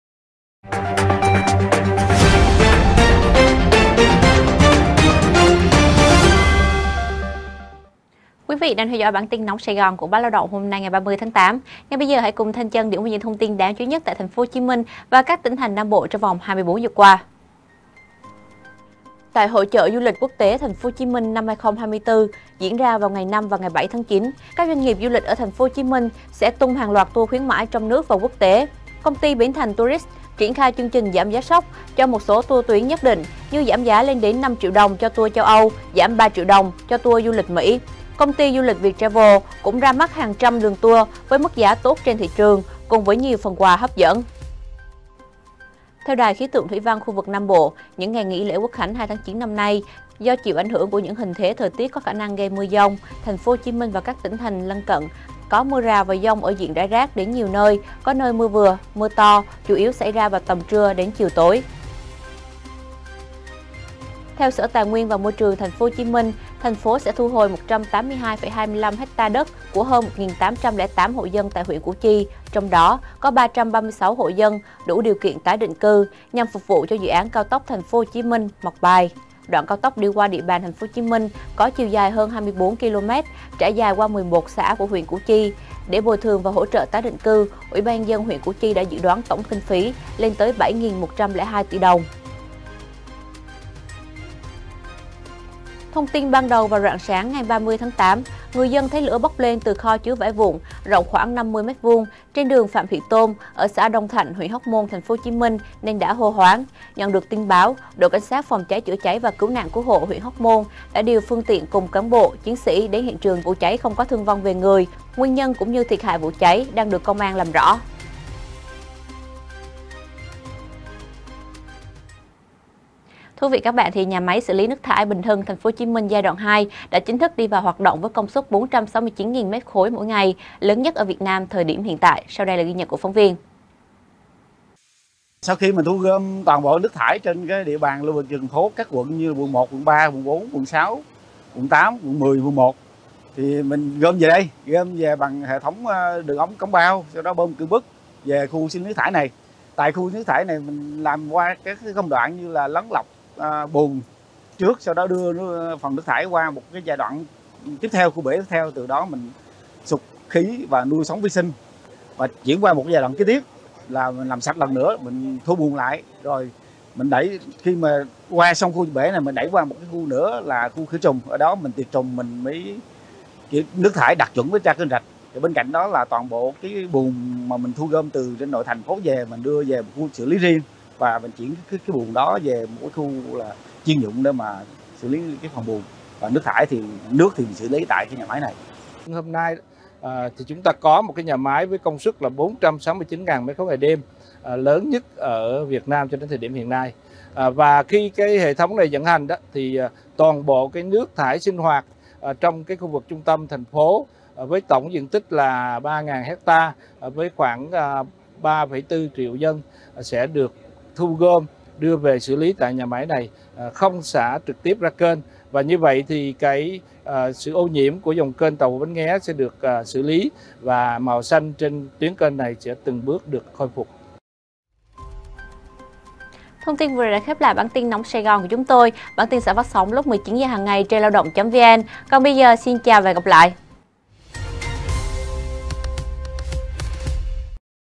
Nóng Sài Gòn: Thu hồi đất 1.808 hộ dân Củ Chi làm cao tốc| Báo Lao Động
BÁO LAO ĐỘNG |Tin tức Nóng Sài Gòn ngày 30.8: Thu hồi 182 ha của 1.808 hộ dân Củ Chi xây cao tốc; Dự báo mới nhất về mưa dông tại Nam Bộ dịp lễ 2.9;...

* Đường dây nóng: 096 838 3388